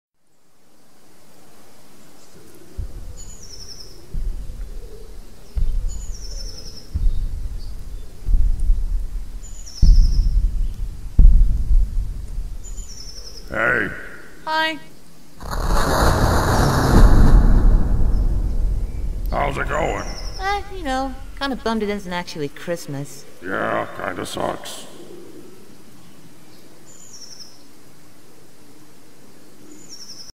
[MMD x DBZA] Goku is bummed out (45 FPS)
This is something I started since last week. I wanted to make this based on one of the DBZA movies from TeamFourStar.

Goku - CrossMMD
Slug - Adun175 (I had to rename the bones and add facials for motion use)
Stage - GuiltronPrime
Skydome - kaahgome
Effects - Scale, BlackOut3, ActiveSmoke, Earthquake